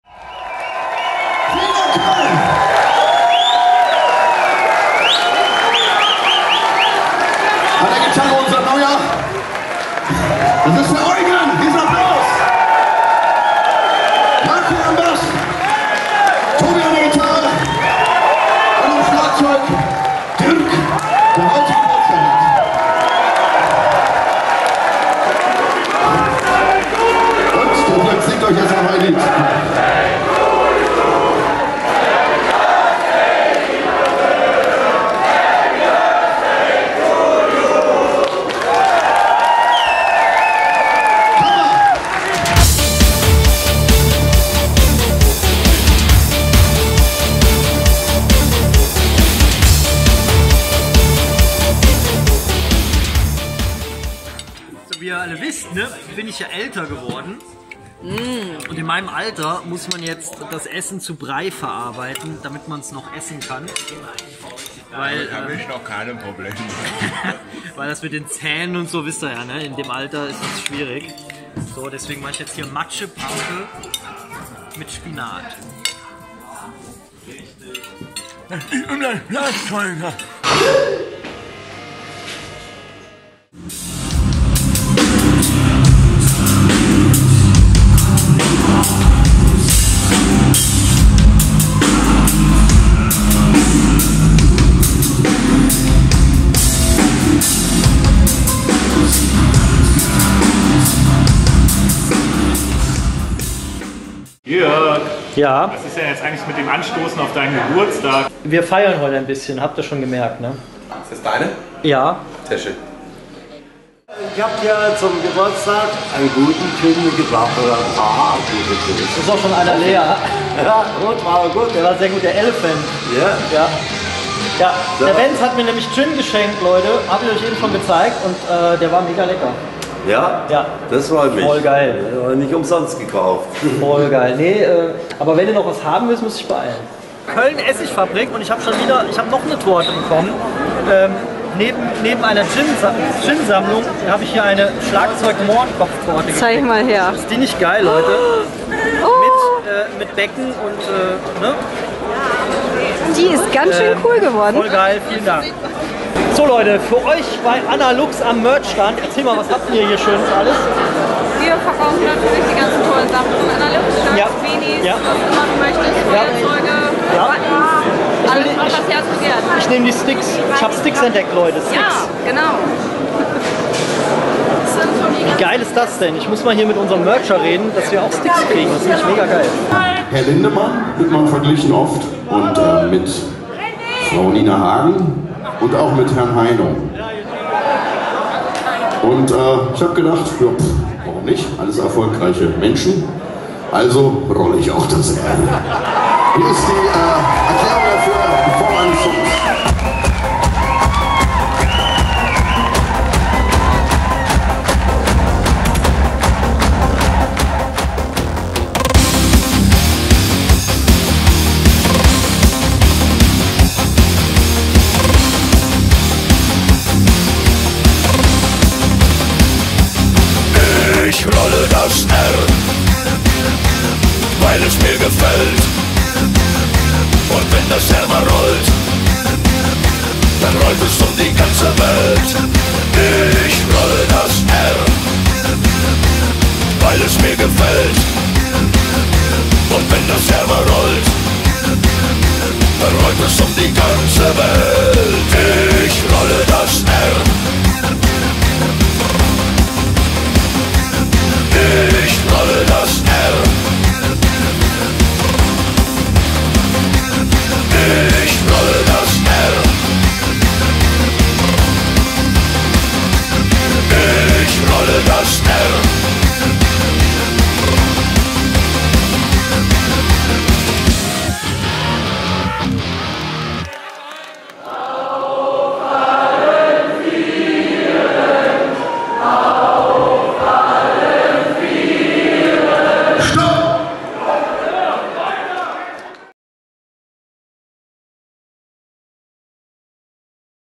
Heldmaschine LIVE+LAUT Tour Tagebuch diary #18 Köln - Coesfeld
Köln: BÄM ! Coesfeld: FEEETT !! Wer von Euch sehen wir zur NACHT DER HELDEN wieder ??? Die wird GEIL !!

Das wird DIE Jahresabschluss-Party !!